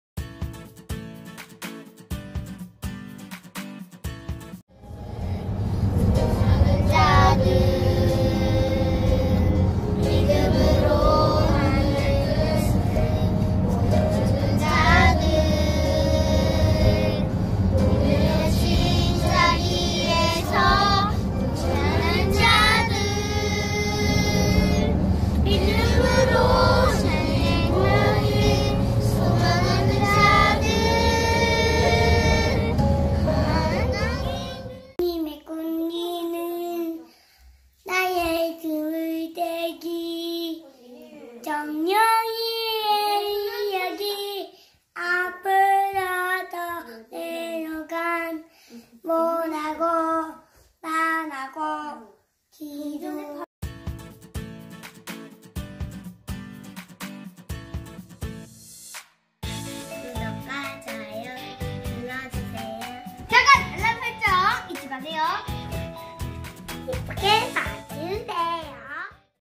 찬양(ccm)_막 따라부르기_어디서든 찬양이 좋은 아이들(ft.가사 신경 안쓰고 즐기는 편)
2018년 겨울~2019년 봄...쯤 영상인듯 해용
우리 아이들은 동요보다 찬양에 익숙해서 그런지
차 안에서도 저렇게 막 따라부릅니다...
엄마 눈엔 하트 뿅~~♡♡